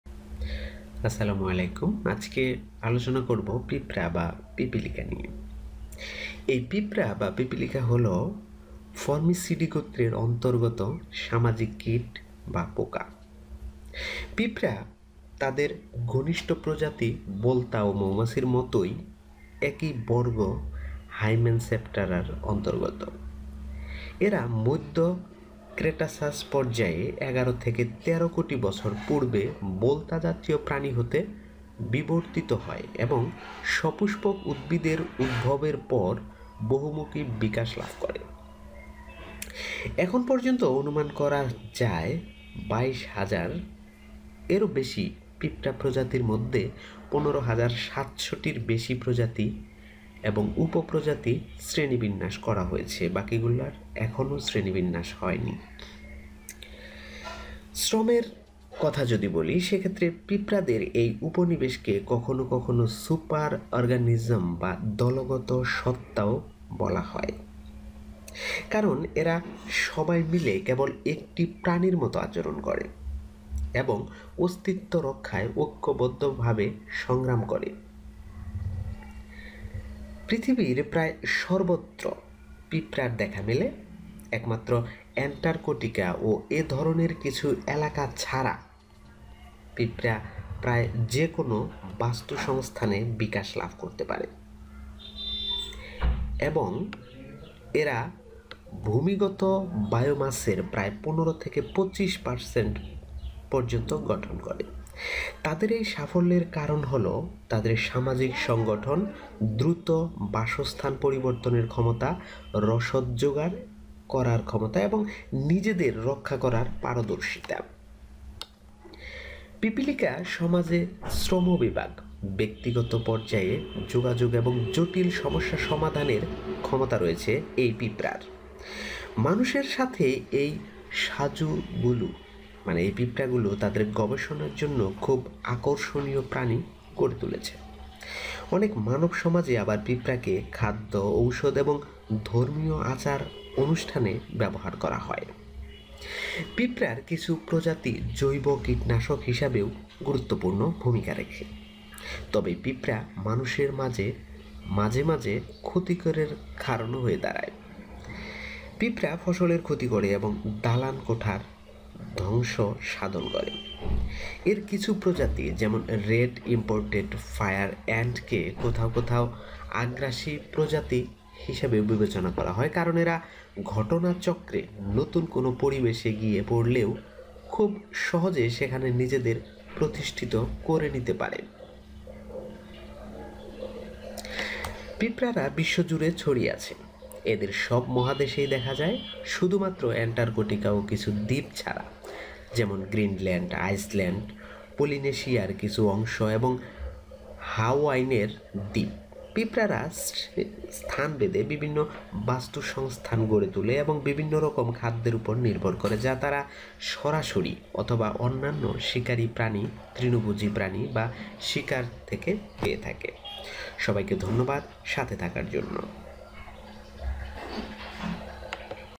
পিপড়া সম্পর্কে কিছু অদ্ভুত তথ্য যা আপনি বিশ্বাস করতে চাইবেন না। Ants are spread all over the world
Ants are social insects belonging to the family Formicidae. ants belong to the same class Hymenoptera as their closely related species of wasps and bees. They evolved from gorse-like animals during the mid-Cretaceous stage between 11 and 13 million years ago and became versatile after the emergence of flowering plants. Of the more than 22,000 ant species estimated so far, more than 15,700 species and subspecies have been classified.

Department of Labor
This colony of ants is sometimes called a super-organism or a group because they all act like one animal and fight unitedly to survive.

Ants are found almost everywhere in the world, except Antarctica and some such areas. Ants can thrive in almost any ecosystem and they constitute about 15-25% of terrestrial biomass. Their success is due to their social organization, ability to change habitats quickly, ability to gather supplies and skills to protect themselves.

Ant societies have division of labor, communication at the individual level, and complex problem solving skills. These similarities to humans make them very interesting animals for research. In many human societies, ants are used in food, medicine, and religious rituals. Some species of ants are also important as biopesticides. However, ants can sometimes cause damage to humans. Ants damage crops and destroy buildings. Some of its species such as the red imported fire ant are considered invasive species in some places. Because even if they happen to fall into a new environment, they can easily establish themselves there.
Phylogenetic position of the Formicidae
The family Formicidae belongs to the class Hymenoptera which includes bees, wasps and sawflies in addition to ants. Ants evolved from a lineage of vespoid beetles. Fossil records show that they existed 15 million years ago, i.e. at the end of the Jurassic period. After flowering plants evolved about 10 million years ago, they became more diverse and achieved ecological prominence about 6 million years ago. In 1966, E. O. Wilson and his colleagues discovered a fossil of an ant from the Cretaceous period, the species Sphecomyrma freyi, trapped in amber. This 8 million year old fossil has features of both ants and beetles. Sphecomerma was probably a surface-dwelling forager, although many believe that ancient ants like Leptanillinae and Martialinae hunted and ate underground.

During the Cretaceous period, some ancient ant species spread to the Laurasian supercontinent (Northern Hemisphere). Compared to other insects, their numbers were insignificant, about 1% of the total insect population. At the beginning of the Paleogene period, ants became dominant due to rapid evolution and speciation. By the Oligocene and Miocene eras, their number stood at 20-40%. About 1 in 10 species found in the Eocene are still present today. 56% of the surviving species are fossilized in Baltic amber (early Oligocene) and 92% in Dominican amber (early Miocene).

Termites also live in termite colonies, sometimes called white ants, but termites are not ants. They are a subfamily of Isoptera and together with cockroaches they form another family Blatoidae. Termites, like ants, consist of social, sterile workers, but their reproductive traits are genetically different. The similarity between the social structure of termites and ants is called convergent evolution, or homologous but independent (not related to each other).[12] Velvet ants look like large ants, but they are In fact, the wingless wife is a fool.

Distribution and Adaptation
Number of species in the region
Neotropics 2,162
Nearctic 580
Europe 180
Africa 2,500
Asia 2,080
Melanesia 275
Australia 985
Polynesia 42
Ants are spread all over the world. They are found on all continents except the Antarctic and some islands such as Greenland, Iceland, parts of Polynesia and the Hawaiian Islands. Ants form different ecosystems depending on the location and depend on different types of food that they eat directly or from other predators, herbivores or prey.